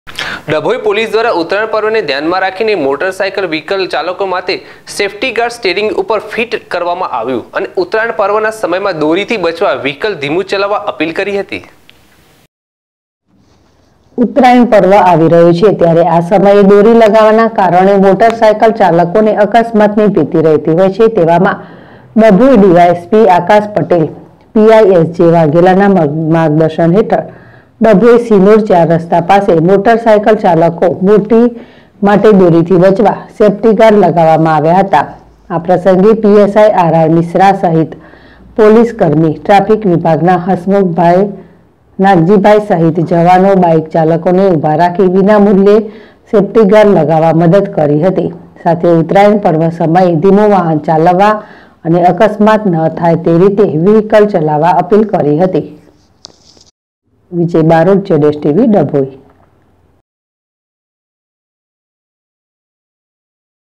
ડભોઇ પોલીસ દ્વારા ઉતરાયણ પર્વને ધ્યાનમાં રાખી મોટરસાઇકલ વિહકલ ચલકોમાતે સેફ્ટી ગાર્ડ સ્ટેરીંગ
ડભોઇ પોલીસ દ્વારા ઉતરાયણ પર્વને ધ્યાનમાં રાખી મોટરસાઇકલ વિહકલ ચલકોમાતે સેફ્ટી ગાર્ડ સ્ટેરીંગ ઉપર ફીટ કરવામાં આવ્યો